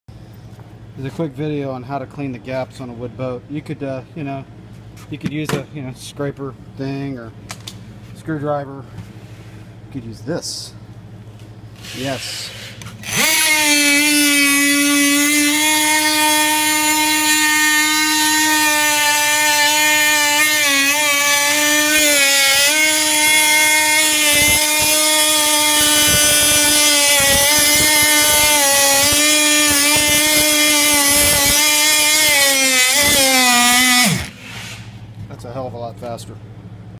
How to clean the board gaps on a big wooden boat.
This saved me days of hand scraping.